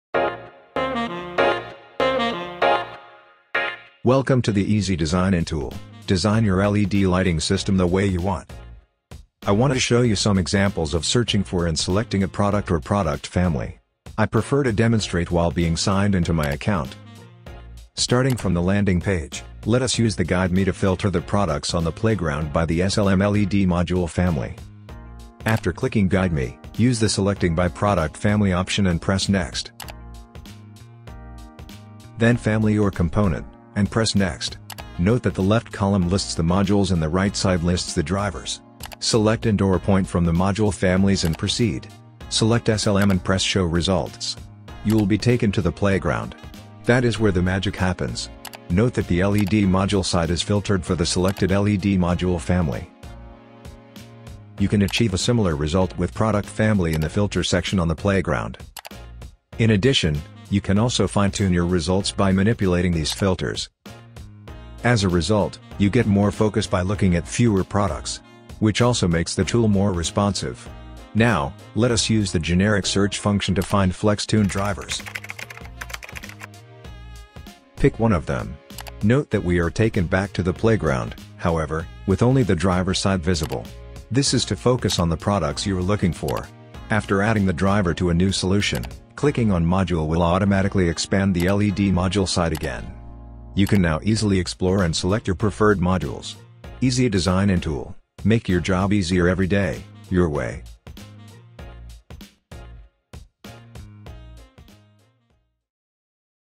Find a Philips product or family with Easy Design-In Tool
Find a Philips product or family in just a few clicks with Easy Design-In Tool!

This online tool allows you to quickly explore our Philips LED lighting components based on your desired specifications. It shows the suitable options from which you can easily assemble your system for your luminaire.

Easy Design-In Tool simulates realistic performance, with the results already validated. Using Easy Design-In Tool will save you a lot of engineering time.